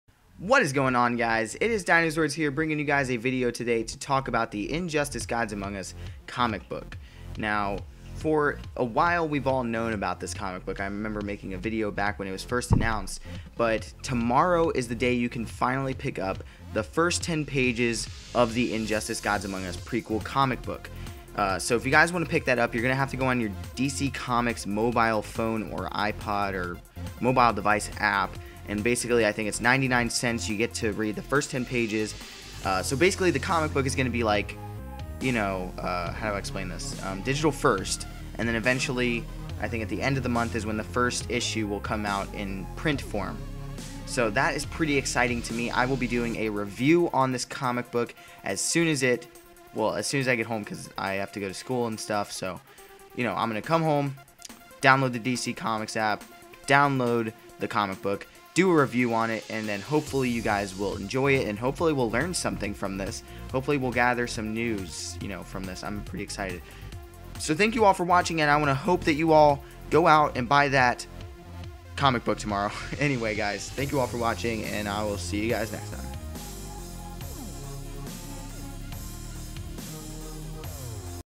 Injustice Gods Among Us: COMIC BOOK TOMORROW!
Yes the first 10 pages of the injustice comic book will be released tomorrow via DC comics mobile app. Make sure to check back here tomorrow for my in depth review of the comic!

For More Injustice Gods Among Us Coverage Check out: Dinozords